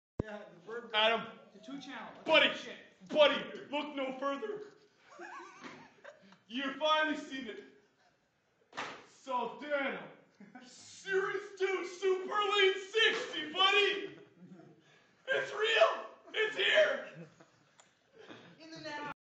New Guitar Head Day! (aka Xmas 2)
Daniel blew up his guitar head while tracking so a new one came in to replace it...and Sandy is pretty excited about it